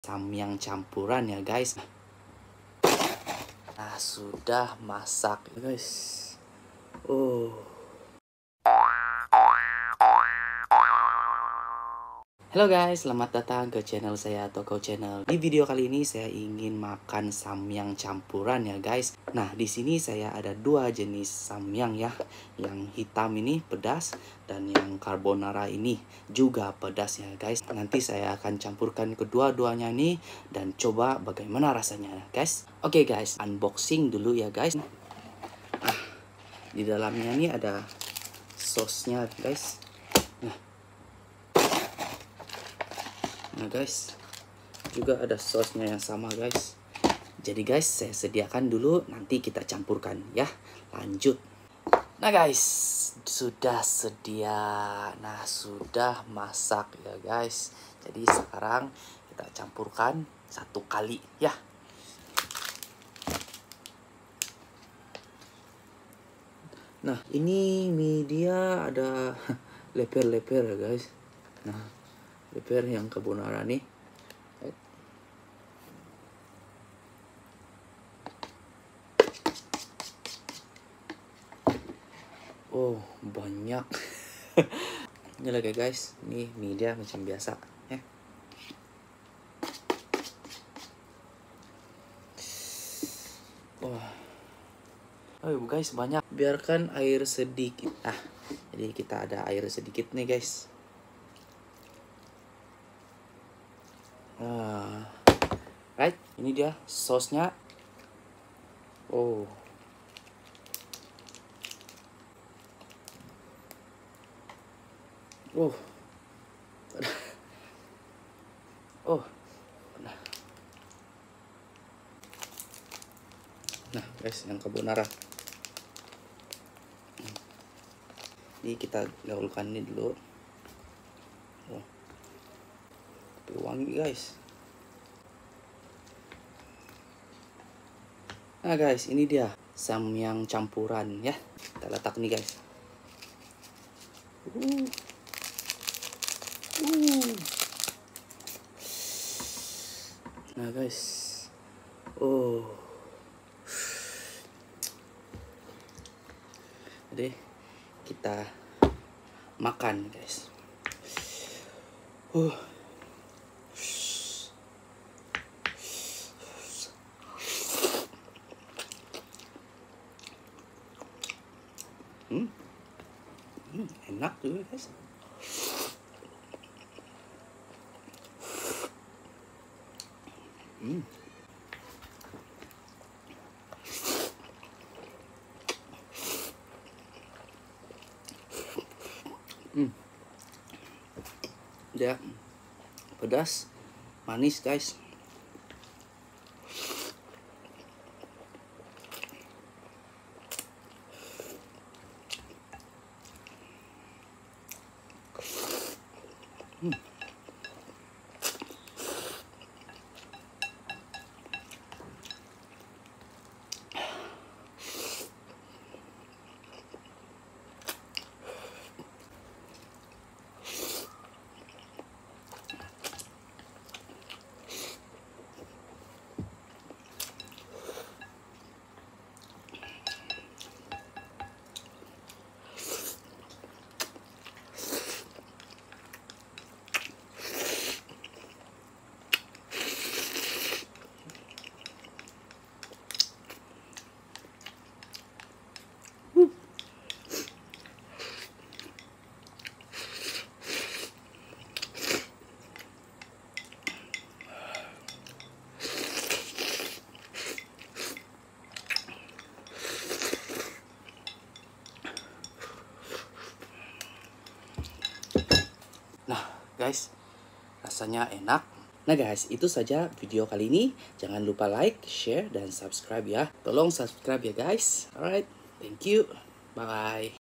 MAKAN / MUKBANG SAMYANG CAMPURAN | SAMYANG PEDAS DENGAN CARBONARA | RASANYA MENGEJUTKAN
Eating, makan, mukbang

Jenis makanan:
Samyang pedas (original)
Samyang pedas (carbonara)

Aku try campurkan dua jenis samyang. Belum pernah cuba, tapi teringin cuba. Mahu tahu bagaimana rasanya, cuba tegok video ini.. Tonton sampai habis ya..

Samyang is one of the most popular food in the world. Who doesn't know samyang. Yes, a noodles from korea. Today I will try 2 kinds of samyang and mix it. I really wanted to taste it. Watch this video and see what my reaction on mixed samyang.

Hello korea from me, Tokou Channel.

Musician: LiQWYD